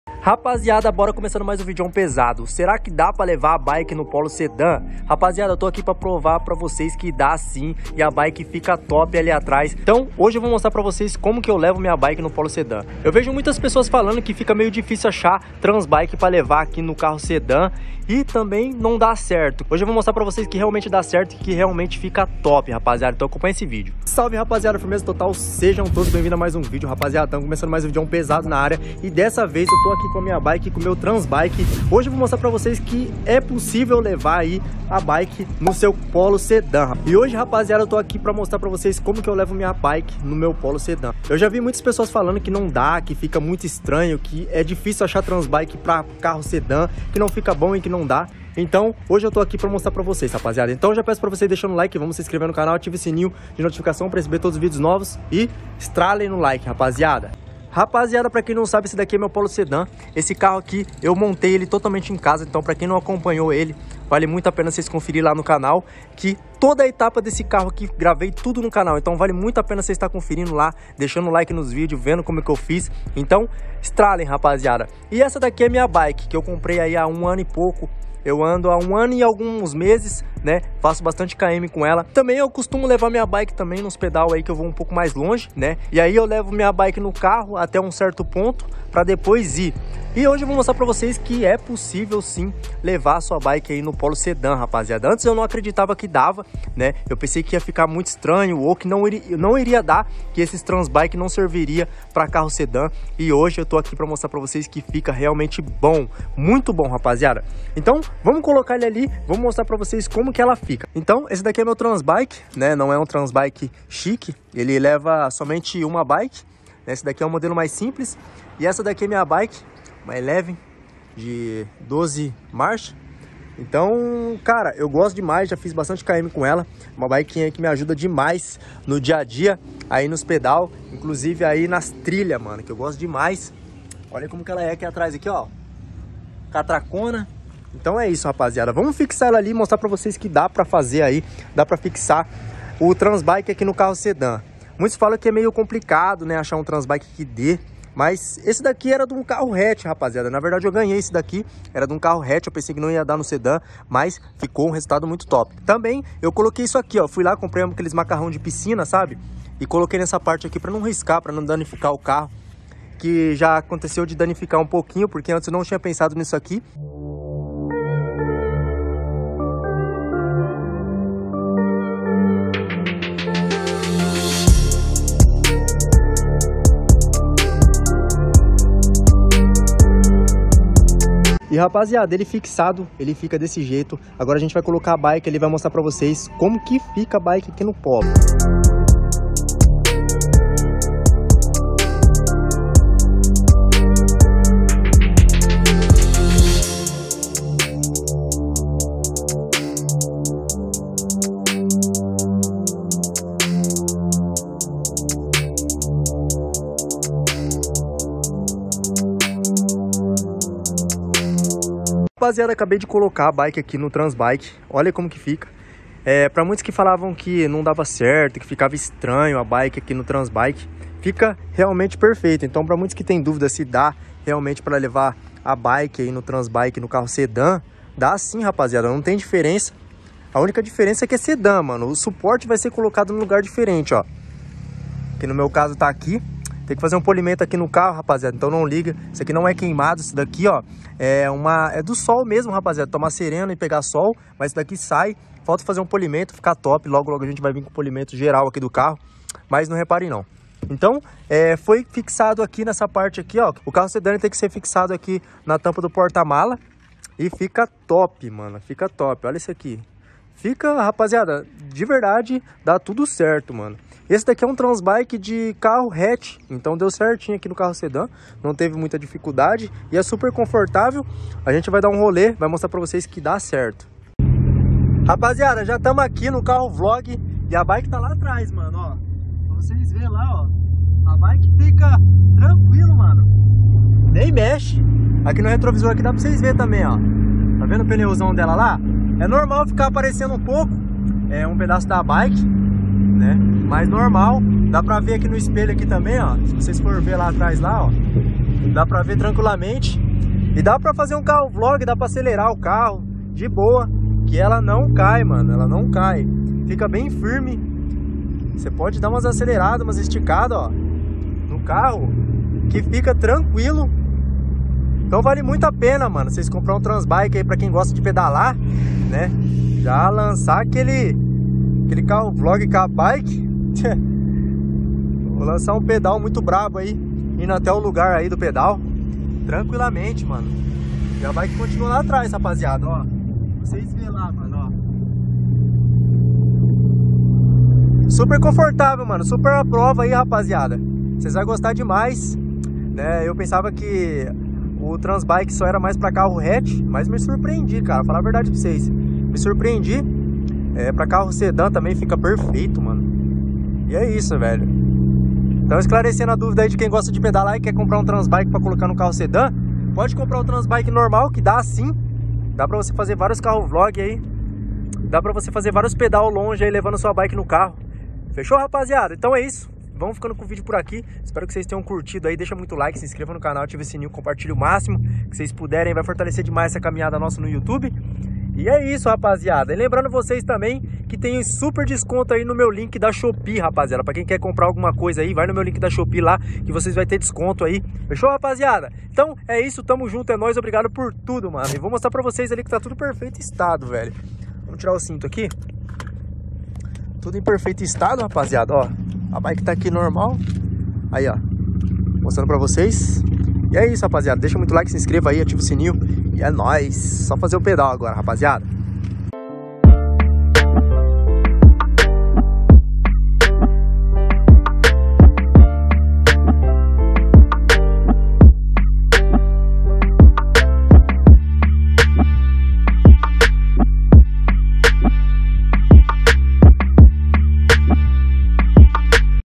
Tem Como Levar A Bike No Polo
Pra muitos que tinham duvida sobre se da pra levar a bike no transbike no carro sedan , inclusive no polo , então decidi fazer o vídeo pra vocês verem espero que curtam

► Carro : POLO SEDAN 2008
► Carro : VOYAGE QUADRADO ANO 85
► Carro : FIESTA SUPER CHARGE ANO 2003
► Carro : FORD KA 2009